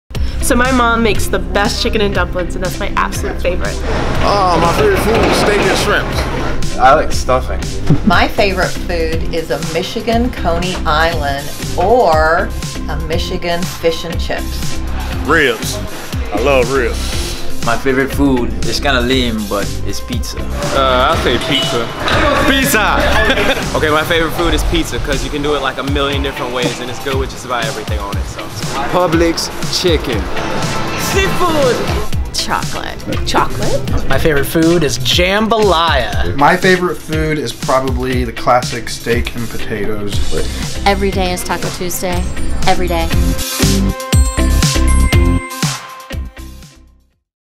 World Food Day 2019
In honor of World Food Day we asked our staff what some of their favorite foods are! It seems that great tummies think alike.